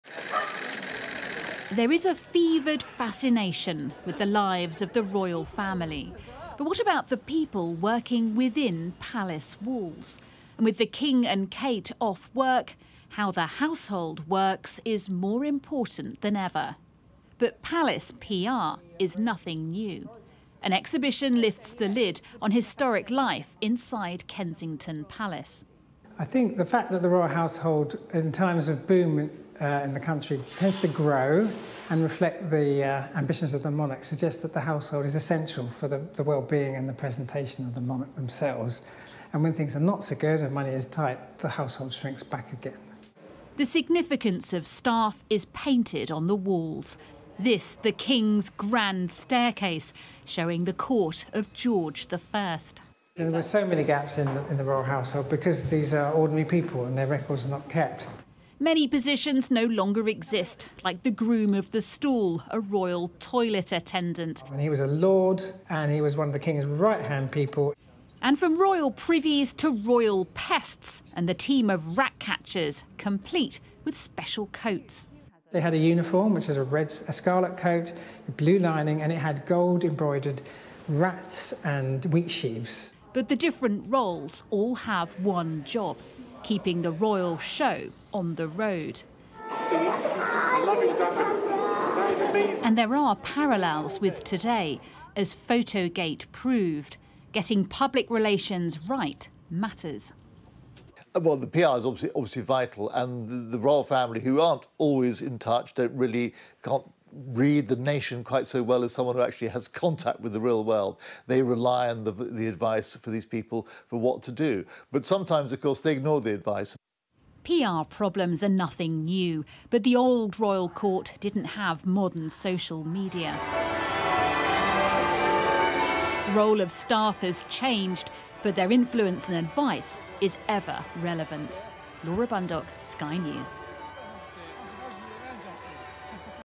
Kensington Palace unveils exhibition on Royal staff from the past | Royal family
With the King and Kate off sick and a recent PR embarrassment, the work of the Royal household is under the spotlight.

Sky News' royal correspondent Laura Bundock looks at a new exhibition, lifting the lid on life in the 19th century Royal household and asking if there are parallels to today.